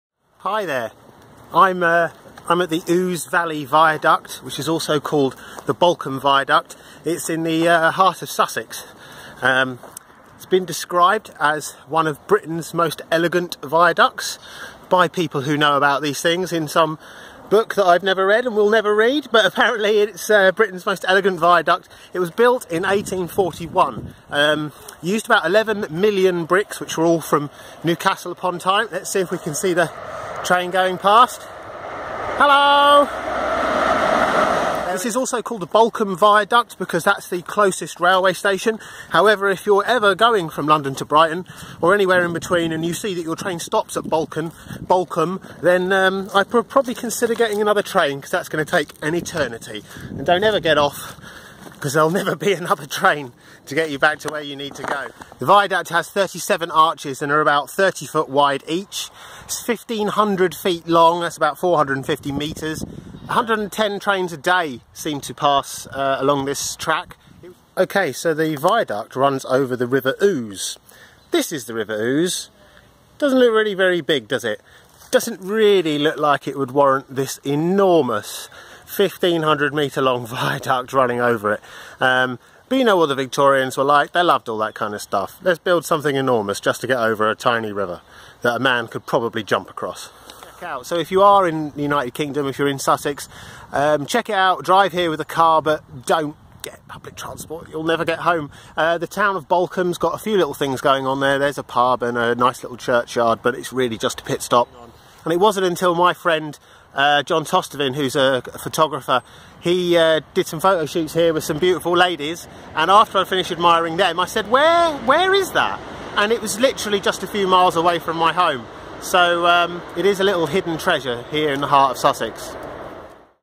Trip to Ouse Valley Viaduct in West Sussex - March 2017 (aka Balcombe Viaduct)
Two minute guide to the Ouse Valley Viaduct (aka Balcombe Viaduct) near the town of Balcombe in Mid-Sussex, United Kingdom. Great Britain's most elegant viaduct, built in 1841; a fine example of Victorian Engineering